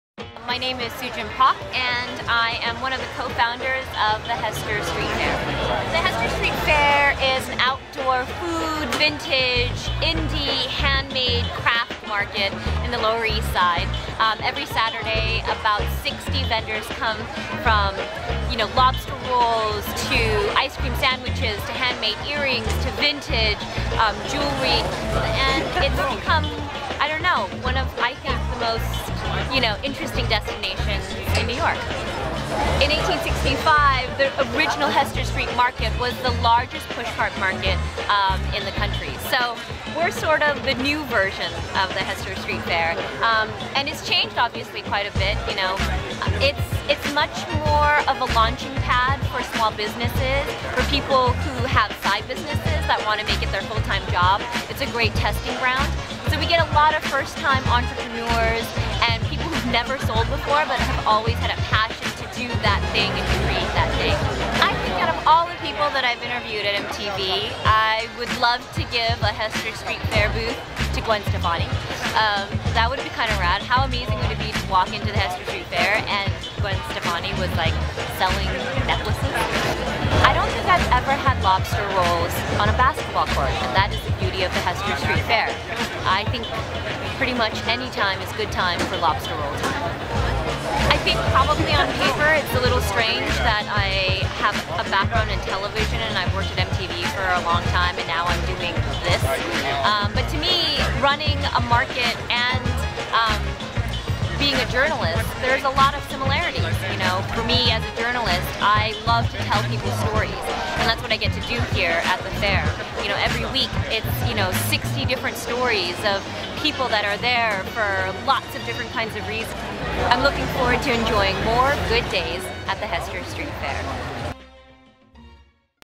Gooddays by Unruly Heir: Suchin Pak - Day in the Life
Gooddays by Unruly Heir goes undercover with mtv personality Suchin Pak at the Hester Street Fair on New York's Lower East Side.